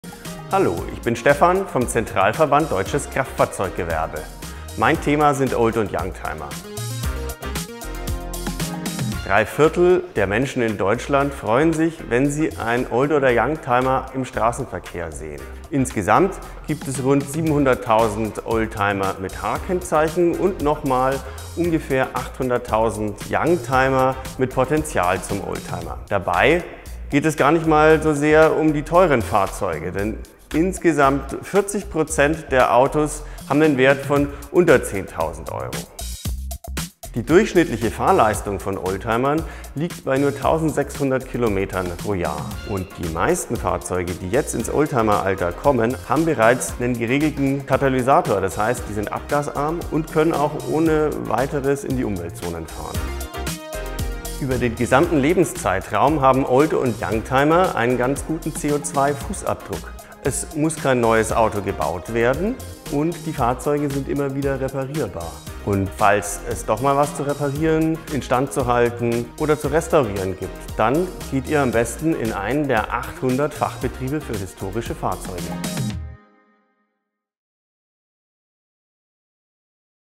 Fachbetriebe für historische Fahrzeuge
Drei Viertel der Menschen freuen sich, wenn sie einen Old- oder Youngtimer im Straßenverkehr sehen. Die jährliche Fahrleistung der Autos liegt durchschnittlich bei nur 1.600 Kilometern. Die meisten Fahrzeuge, die jetzt ins Oldtimeralter kommen, haben bereits einen geregelten Katalysator. Außerdem sind besonders lange genutzte Fahrzeuge nachhaltig. Sie sind bereits produziert und sie können immer wieder repariert werden.
Mehr Infos dazu und zu den 800 Fachbetrieben für historische Fahrzeuge unter kfzgewerbe-oldtimer.de